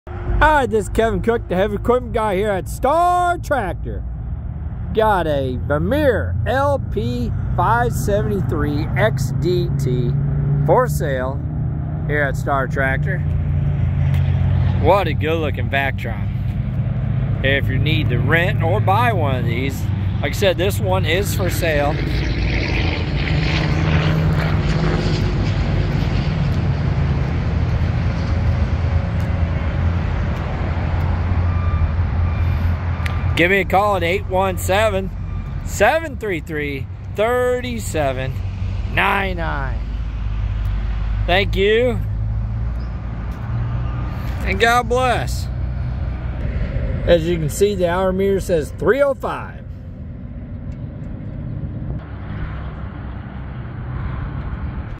Unleash The Power Of The 2023 Vermeer Vac-tron Lp573xdt Trailer (ms-24) - For Sale Now
This 2023 Vermeer Vac-Tron LP573XDT is ready for the job. With only about 305 hours on it, this vacuum trailer is like new.

Powered by a 24 HP Kohler diesel engine, it features a 580 CFM vacuum pump and a 500 gallon debris tank with hydraulic tilt and door. Dual 100 gallon fresh water tanks feed the 3000 PSI at 4 GPM high pressure water system. A 30 gallon fuel tank provides extended runtime. With a 12,000 lb GVWR, this trailer can handle big jobs.